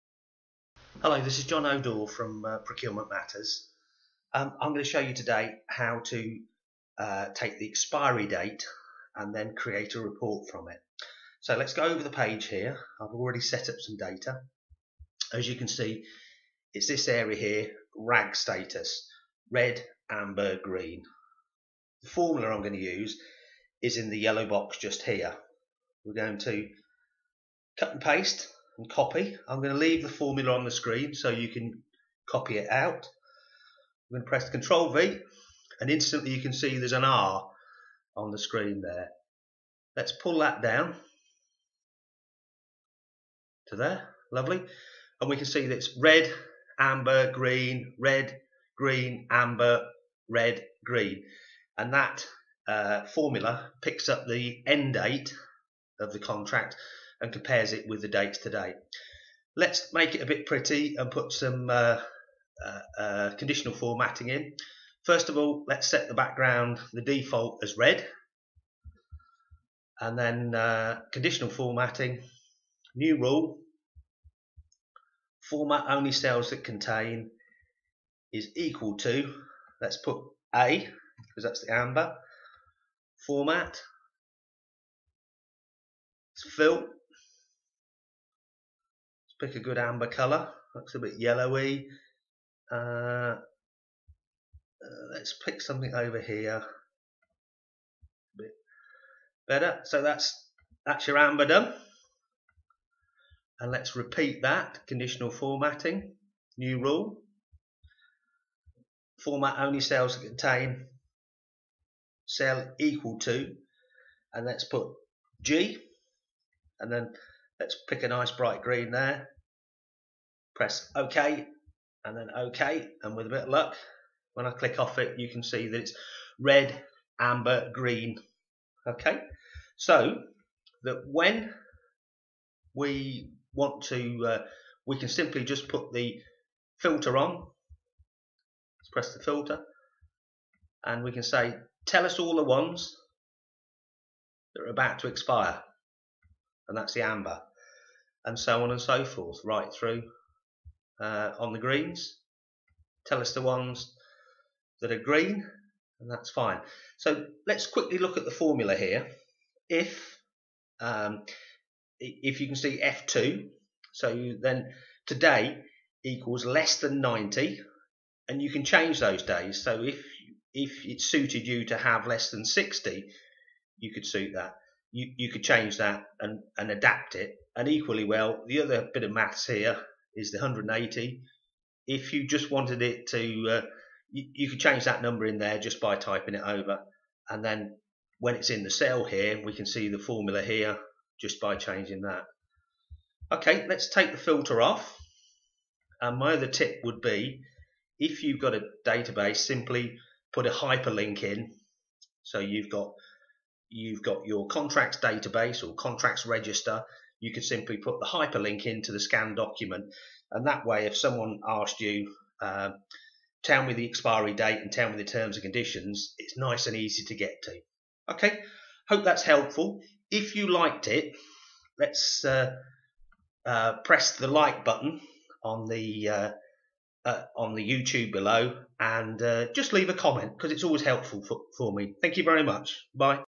Procurement Matters. Keeping Track of the Expiry Date of Contracts
Contracts Register, how to automatically generate a report based on the expiry date of a contract using Excel. Once this report is generated it compares the expiry date to today's date and gives a simple Traffic Light Status, Red, Amber or Green. This is useful when managing the workload of any Procurement Team